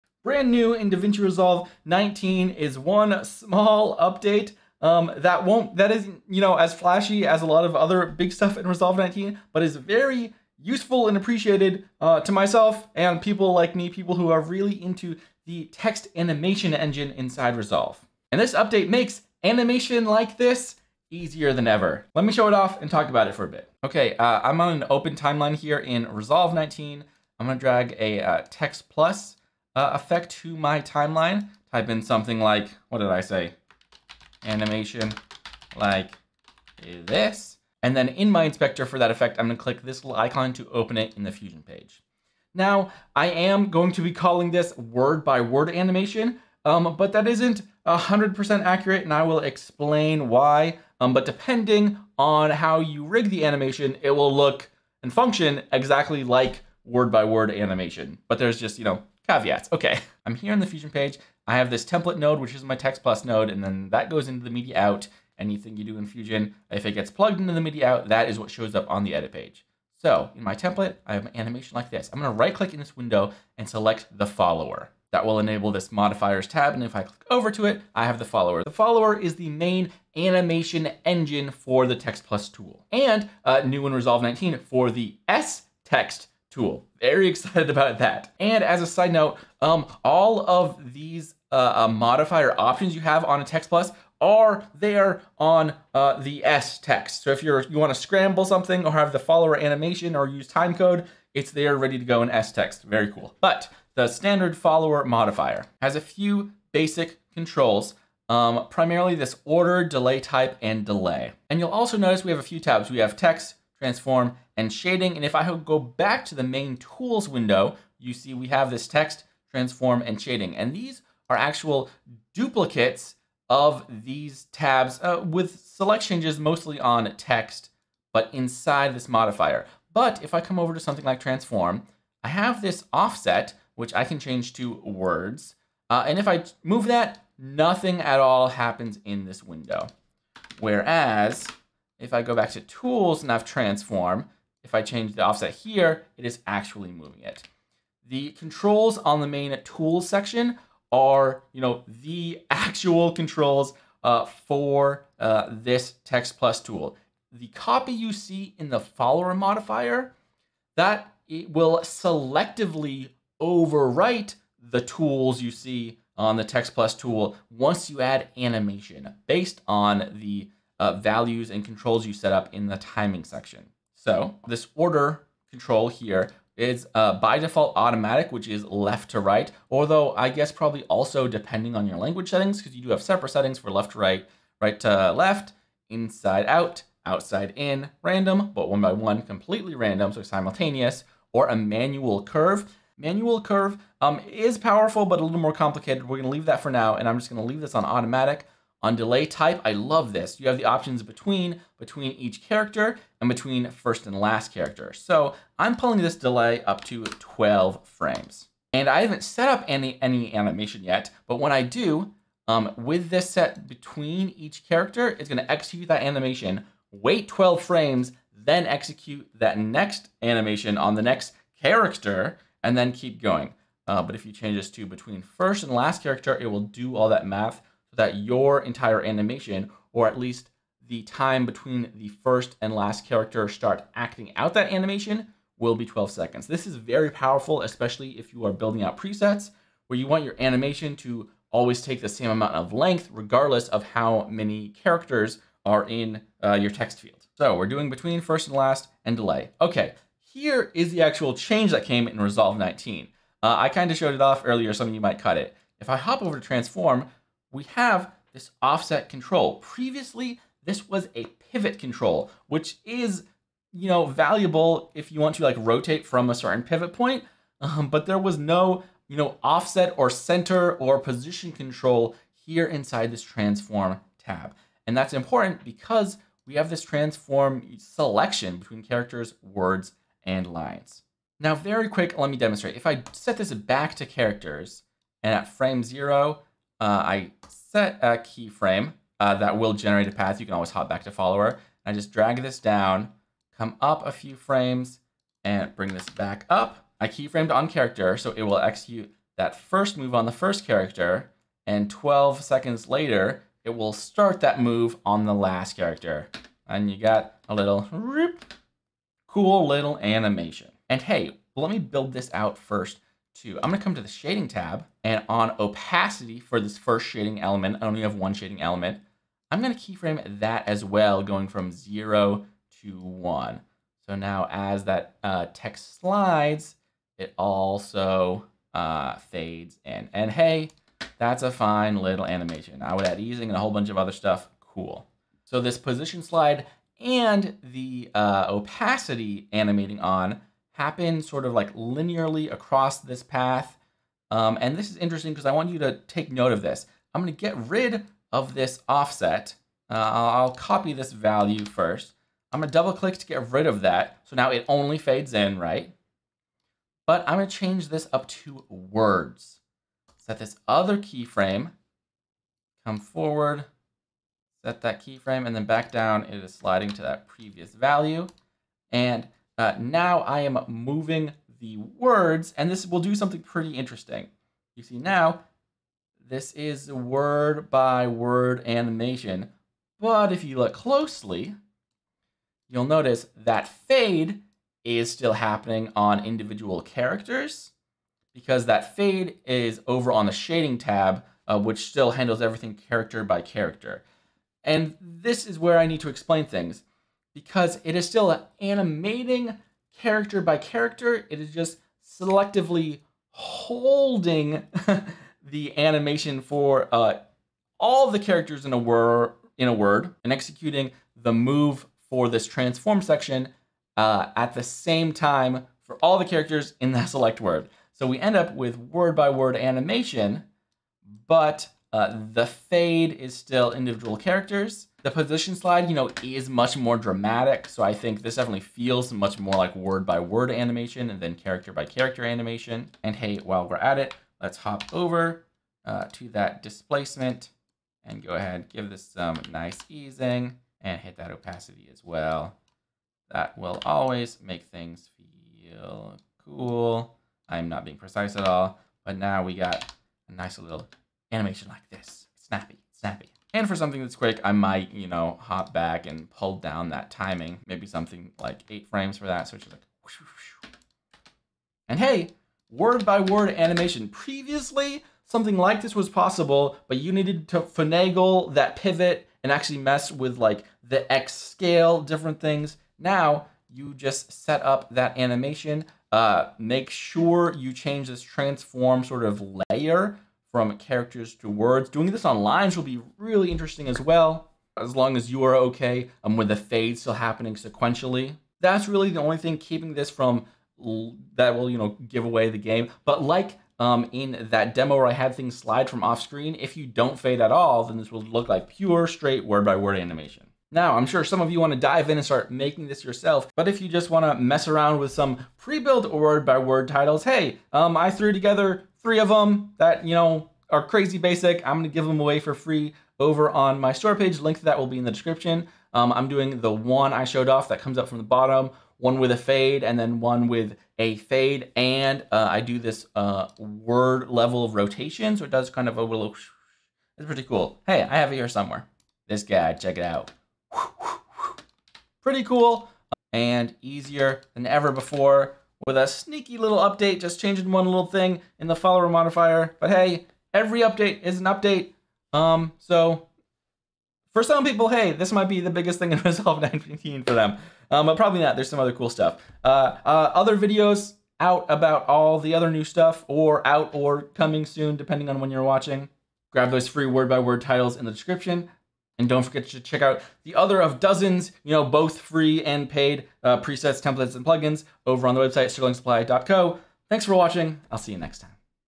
I've Been Waiting for This Text Animation Update! DaVinci Resolve 19
Word By Word Animation in DaVinci Resolve is easier than ever thanks to one small change in DaVinci Resolve 19!